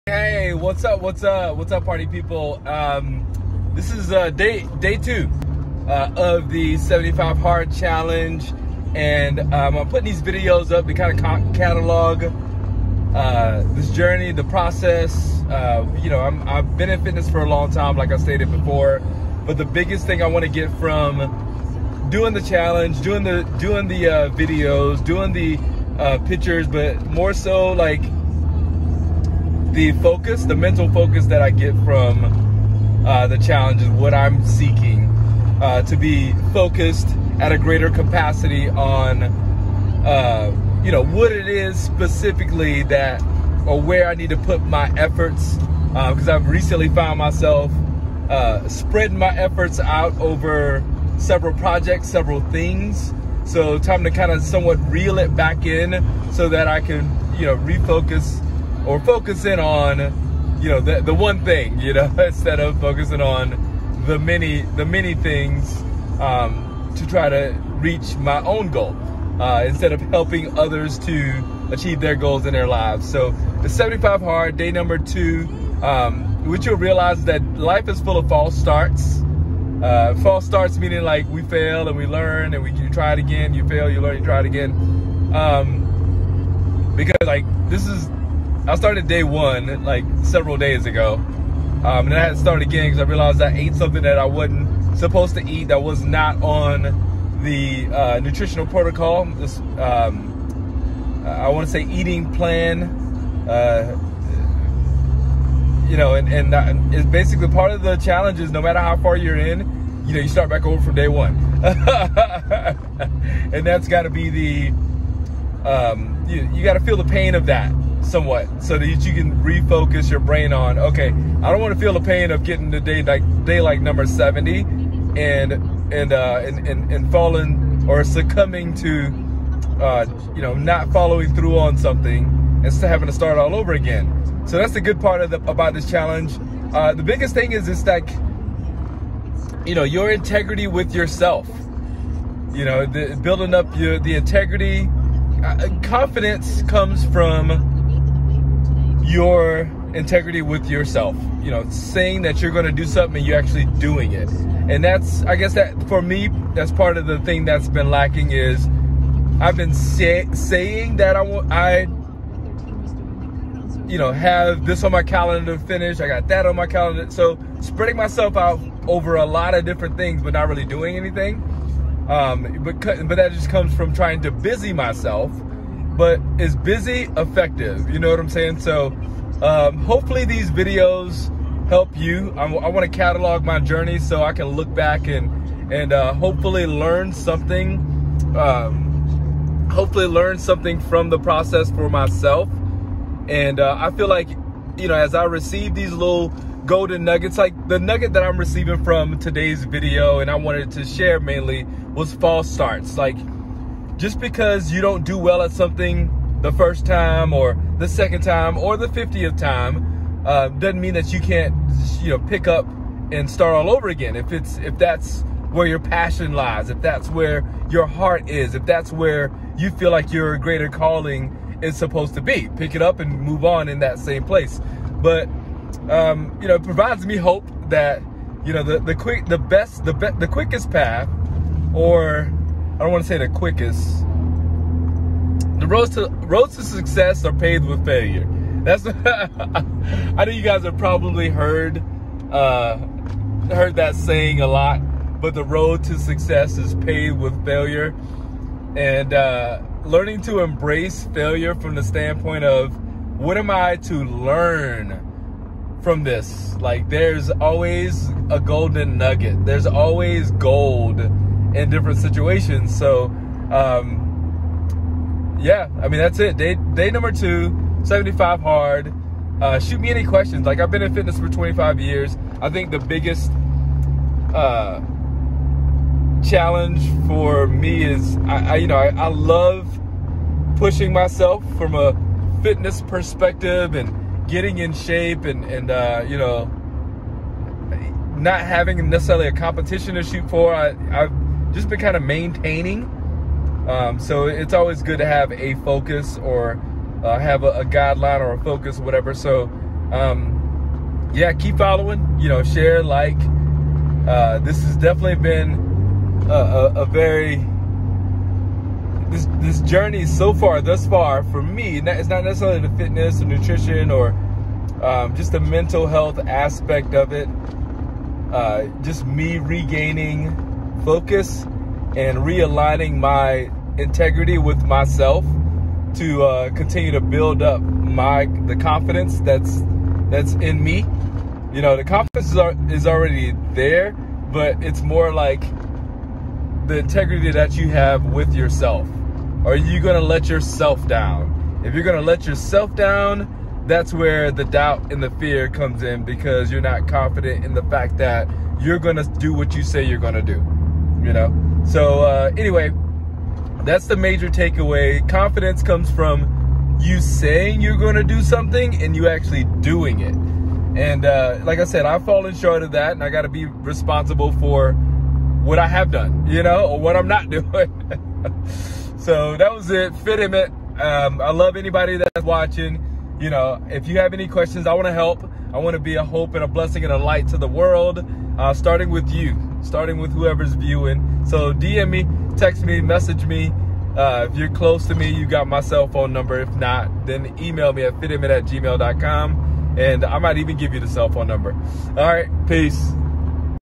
Day 2 And I Failed #75 Hard Challenge
Its Day 2 and Guess what?  I Failed 🤷🏽‍♂️

Documenting 75 days here on YouTube.  I will be sharing some of my workouts. My progress photos and my insights. So excited that Andy Frisella started this challenge!  Looking to regain some focus!  I’ve been in fitness and helping others for 20 years and I’ve competed as a pro bodybuilder but needing more of a mental push right now.

“Helping You Achieve The Best You Possible”